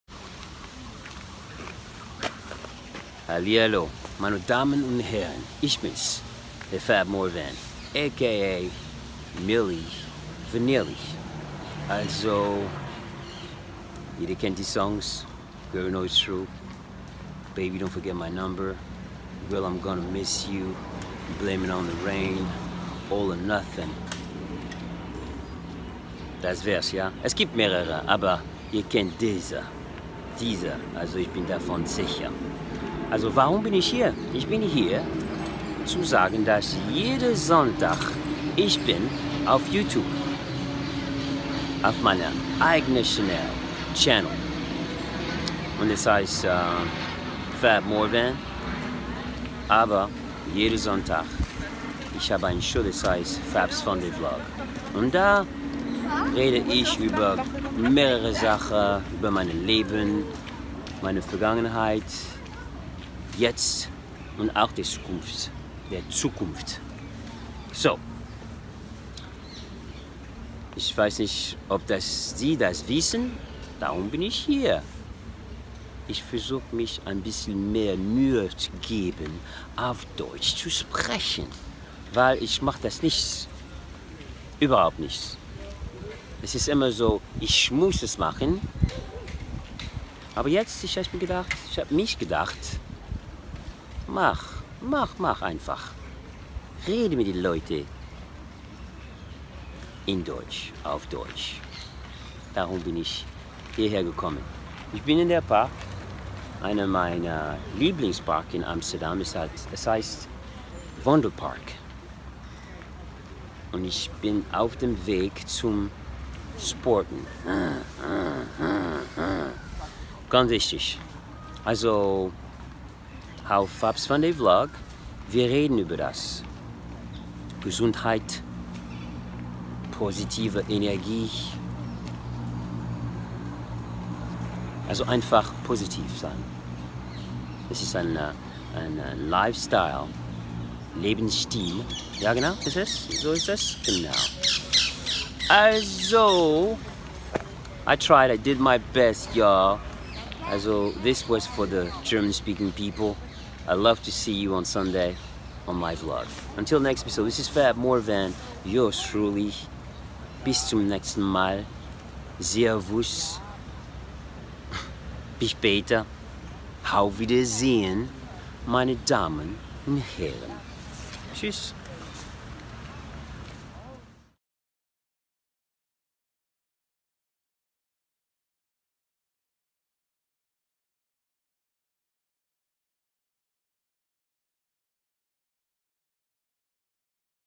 AUF DEUTCH FAB MORVAN MILLI VANILLI “Speaking German Leute”
Hallo!  I have habe meine best getan hoffe ich.
FAB’S FUNDAY VLOG AM SONNTAG. JOIN ME JOIN THE LOVE.